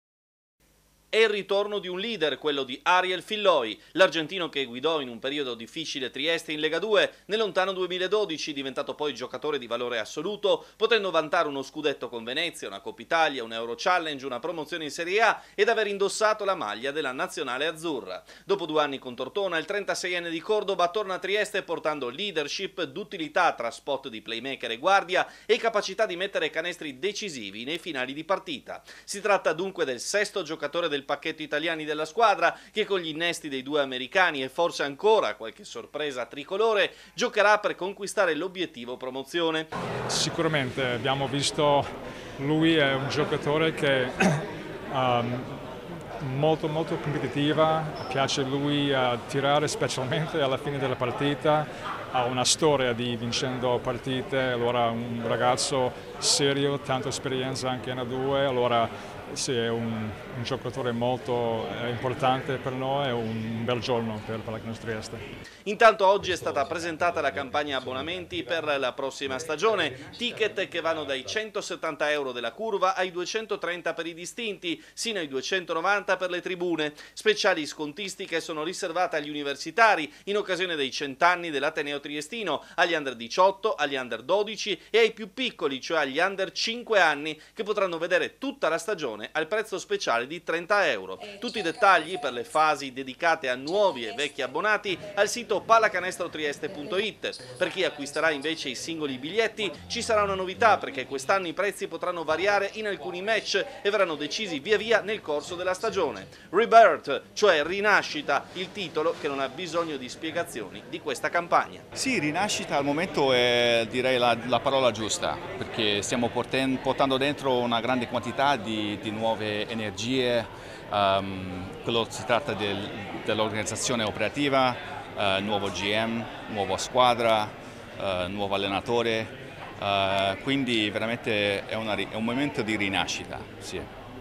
BASKET: TORNA IL LEADER FILLOY, PRESENTATA LA CAMPAGNA ABBONAMENTI | 18/07/2023
TG sera 18/07/2023 - TRIESTE - Grande giornata per la Pallacanestro Trieste fra l'annuncio dell'accordo con l'italo-argentino Ariel Filloy, che torna a Trieste a distanza di 11 anni, e il varo della nuova campagna abbonamenti per il campionato di Legadue.  - Intervistati MICHAEL ARCIERI (GENERAL MANAGER PALLACANESTRO TRIESTE), RICHARD DE MEO (PRESIDENTE PALLACANESTRO TRIESTE) (Servizio di Marco Stabile) - Segui Telequattro anche sul digitale terrestre! Visita il sito